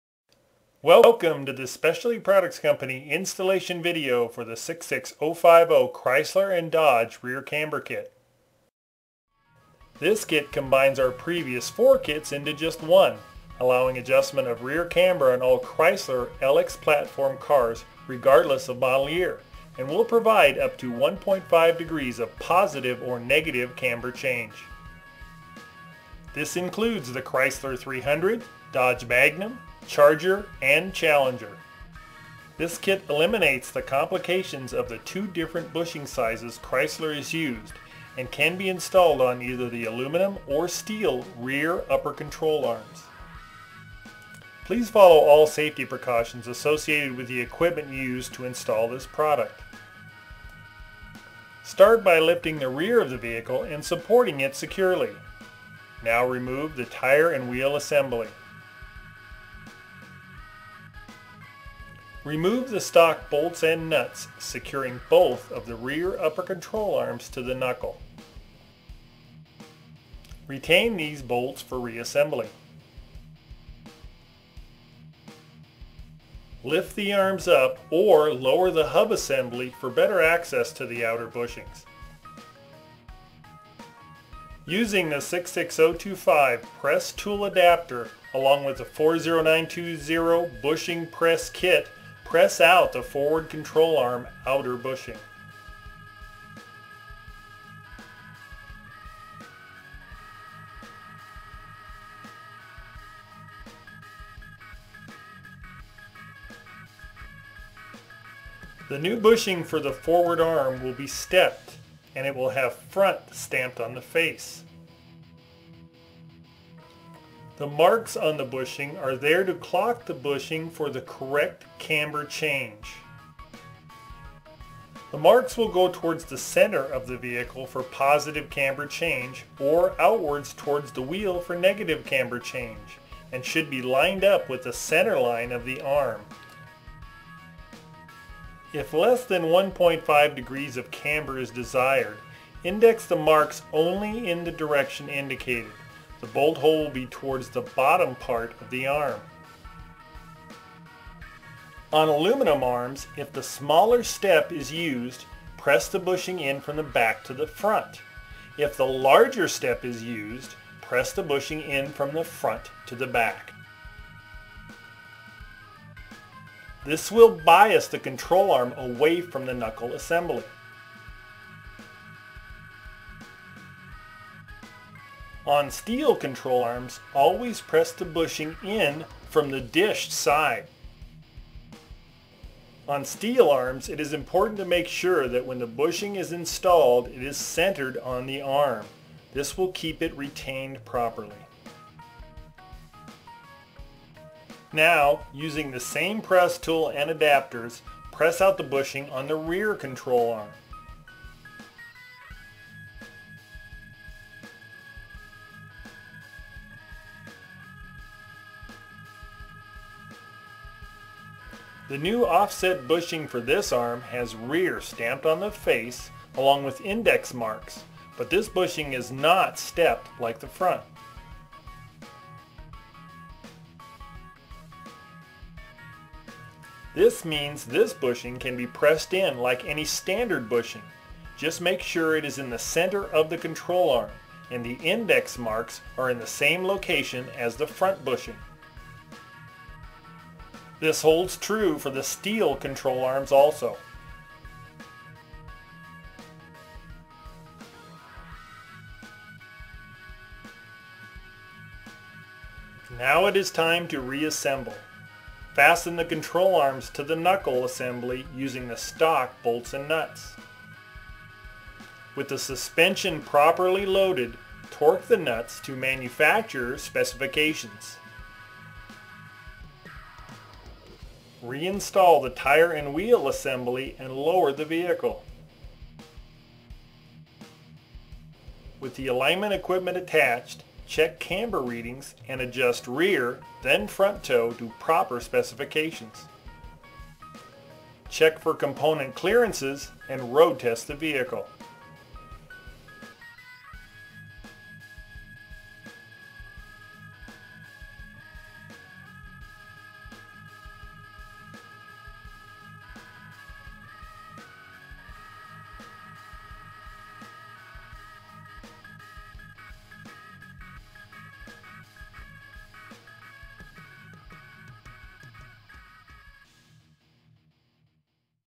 66050 - LX Platform Rear Camber Bushings - Specialty Products Company
This kit combines our previous four kits into one. Allowing adjustment of rear camber on the Chrysler LX platform cars regardless of model year and the complication that Chrysler used two different bushing sizes over the years.
These "set-at-install" bushings can be clocked to provide up to +/-1.5° of adjustment by replacing the outboard bushings on both upper control arms of all Chrysler LX-based cars regardless of aluminum or steel arm construction. By using the #66025 adapter along with the #40910 bushing press, these bushings can be installed without removing the control arms.

Rear Adjustment Range:

Camber +/-1.5°

Install Time: .6 hr/side

Required: 1 kit per wheel